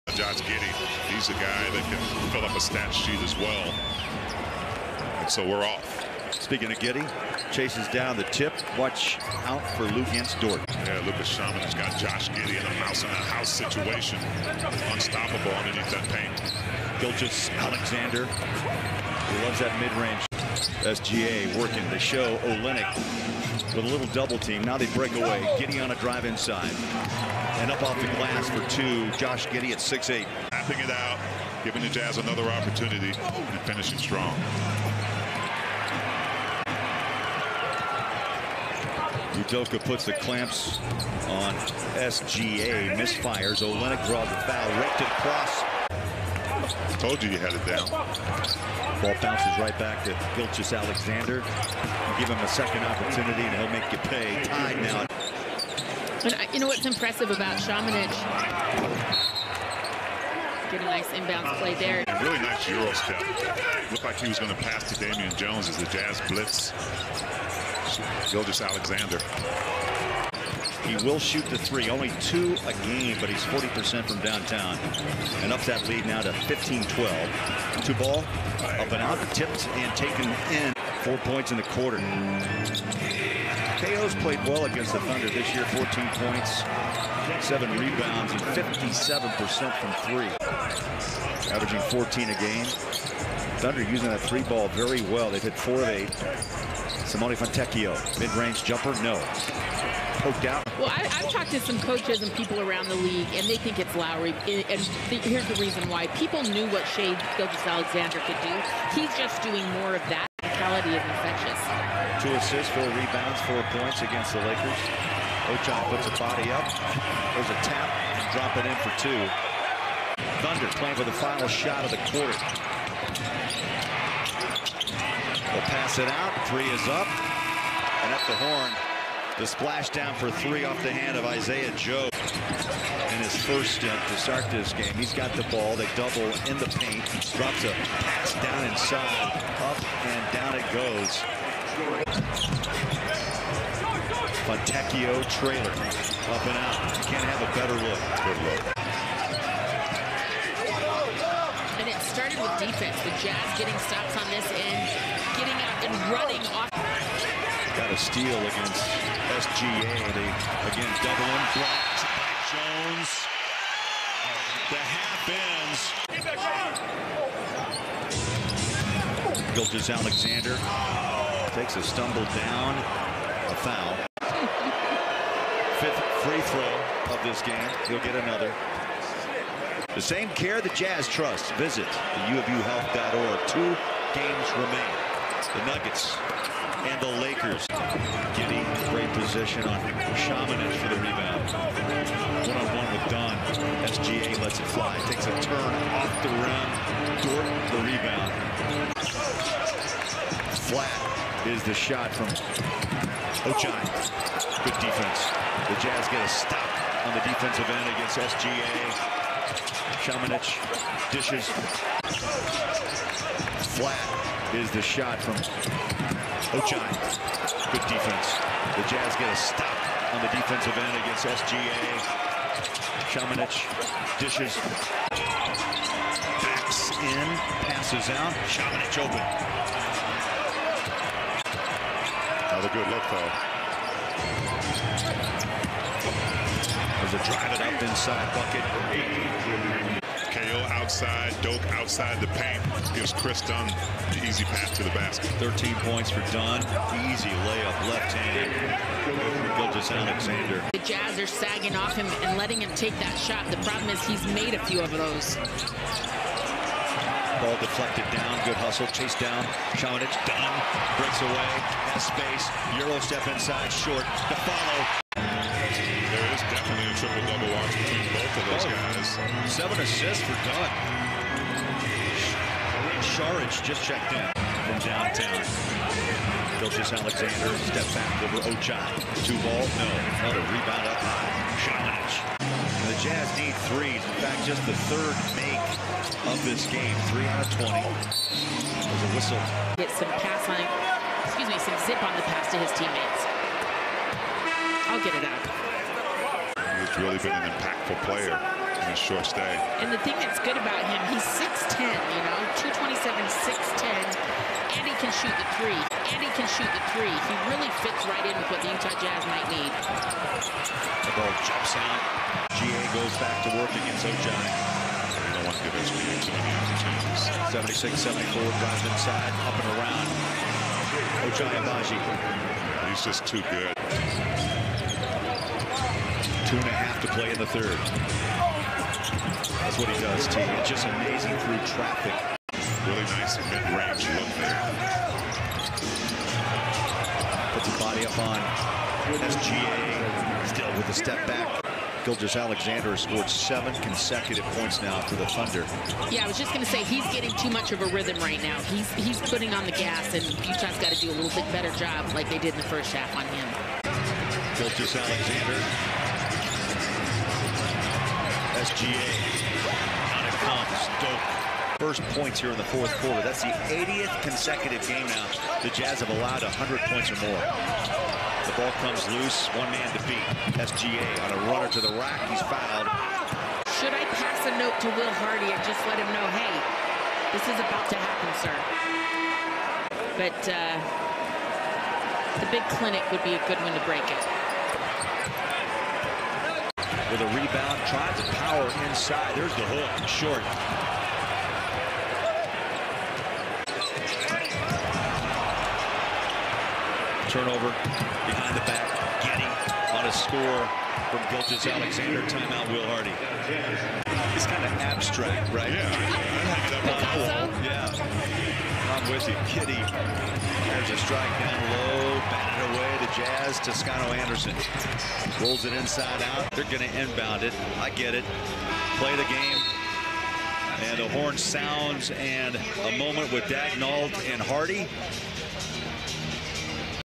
Shai Gilgeous-Alexander 22 pts 7 reb 7 ast vs Utah Jazz | 2023-04-06
Shai Gilgeous-Alexander Highlights from Oklahoma City Thunder vs Utah Jazz on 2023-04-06.

Player Stats:
37:43 mins
22 Points
7 Rebounds (2 Offensive)
7 Assists
1 Steals
0 Blocks
1 Turnovers
7/19 Field Goal
7/10 Free Throw